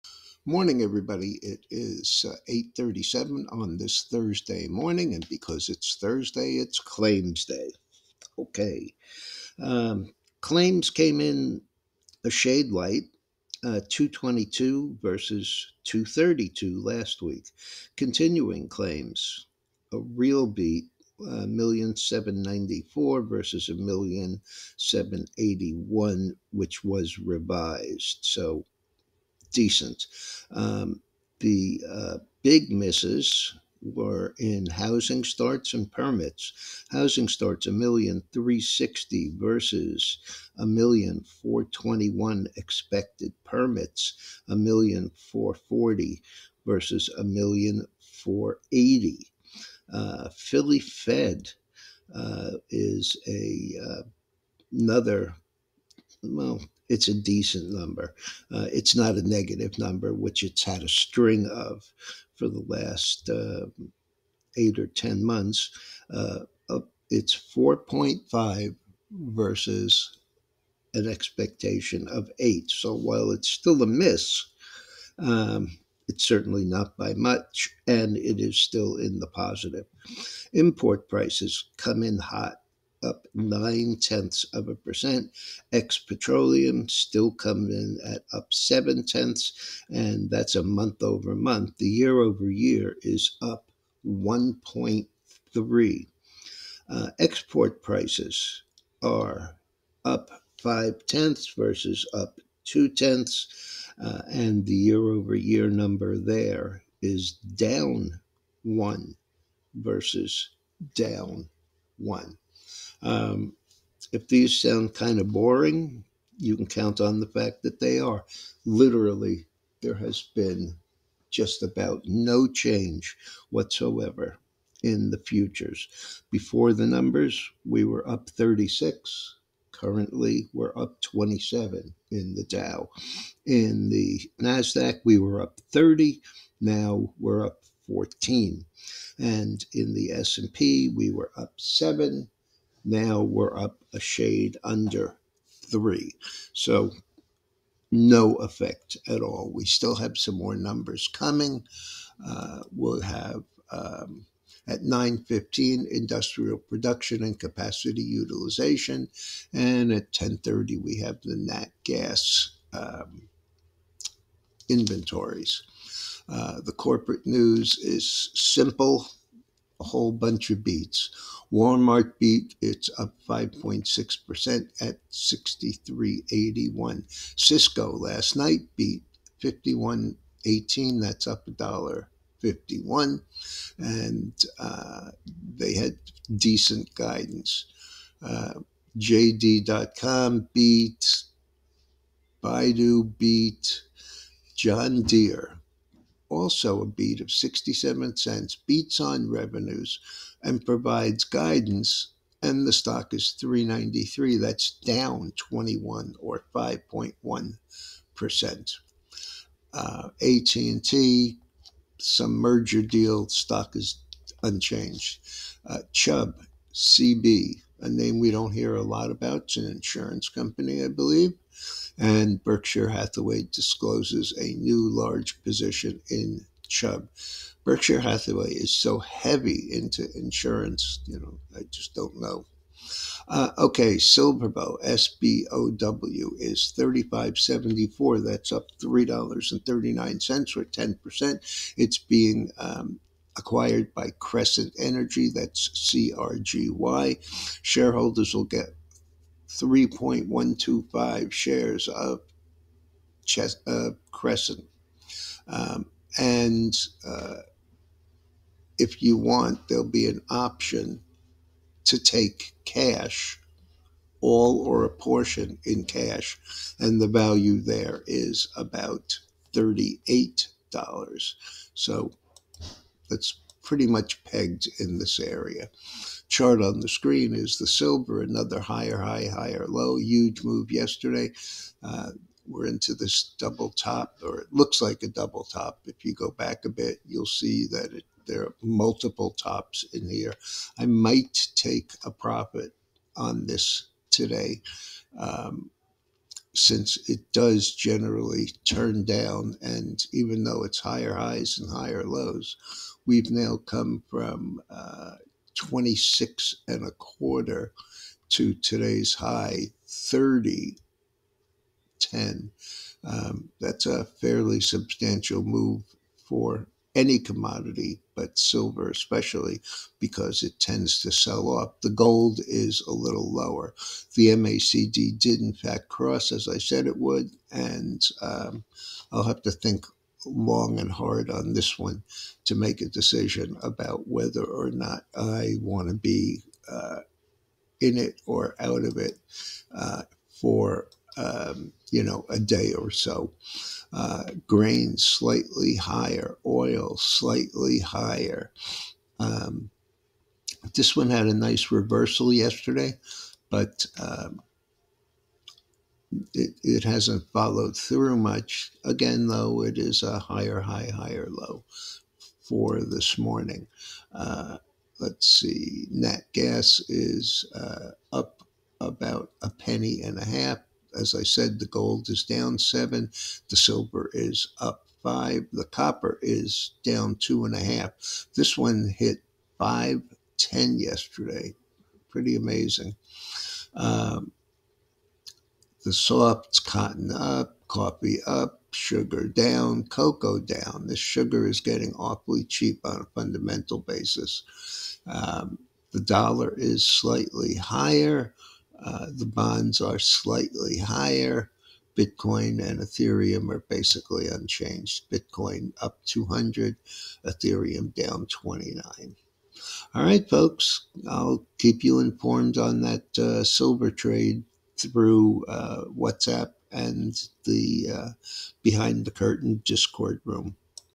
ECONOMIC NEWS LEAVES FUTURES UNCH., WMT, CSCO, DE BEAT, BRKA BUYS CB, CRGY BUYS SBOW, METALS MIXED !
PRE-OPEN COMMENT
VIDEO #2170